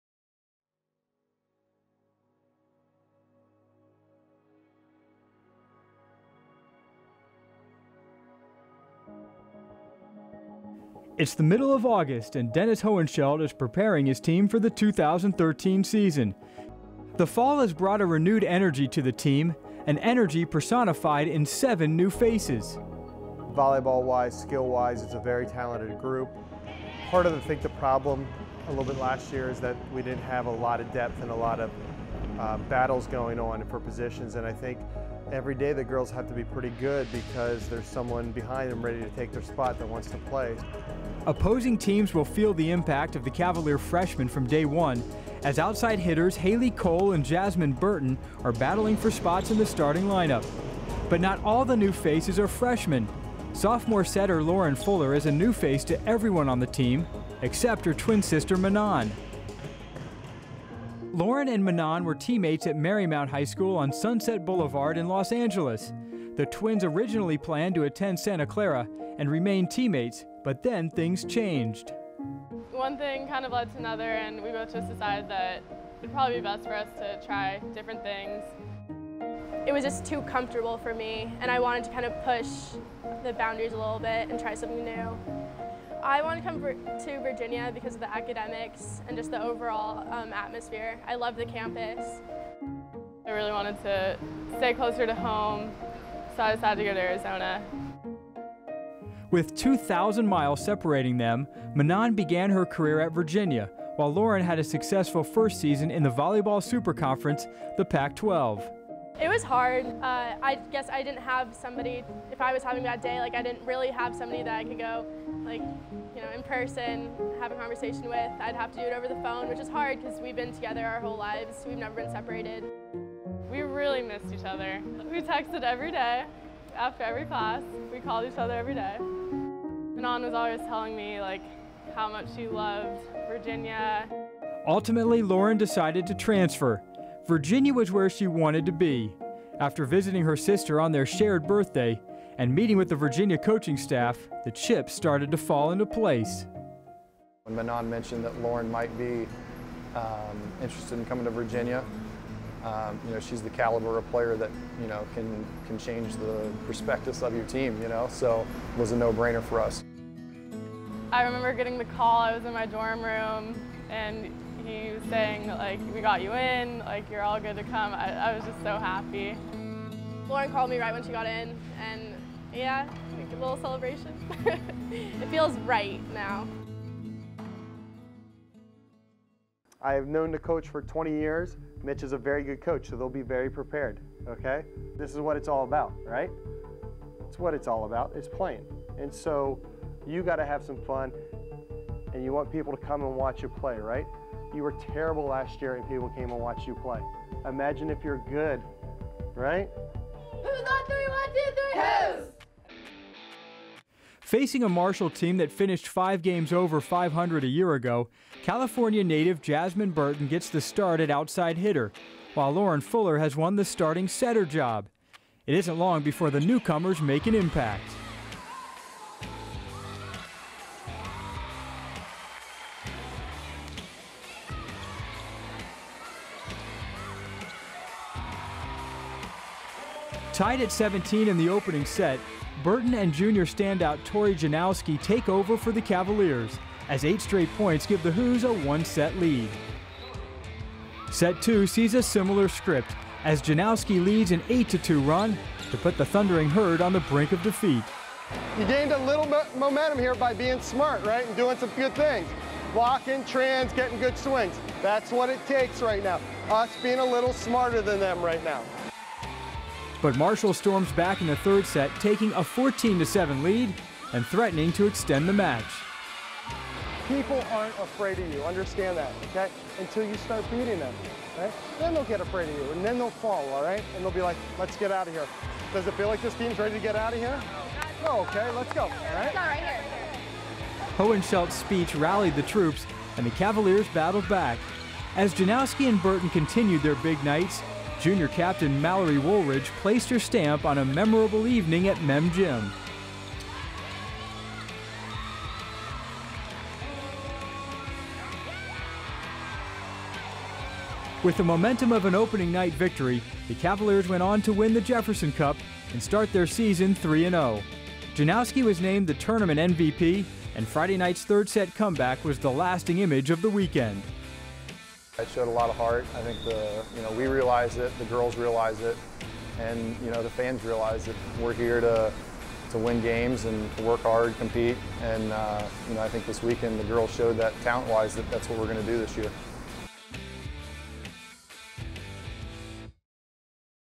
CUE: Volleyball - Opening Weekend
Chasing Uncompromised Excellence goes behind the scenes for the season's opening weekend.